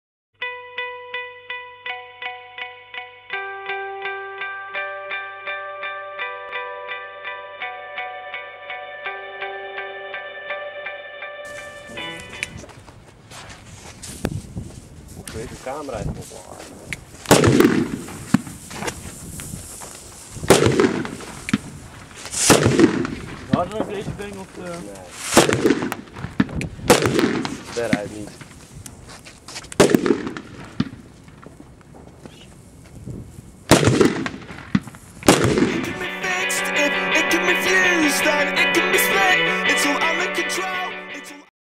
La Bomba (Brothers Pyrotechnics) Oud en Nieuw 2011/2012 (Xtremepyro)
La Bomba Versie 2009 (Brothers Pyrotechnics) Oud en Nieuw 2011/2012. Geschoten met oud en nieuw. Eerst een groene vlam en daarna de knal. De knaller bevat 2.5 gram flash kruit en 0.5 gram flash kruit voor de vlam. Totaal 3 gram flash kruit. Ze zitten met 20 stuks in een pakje en een karton bevat 100 pakjes.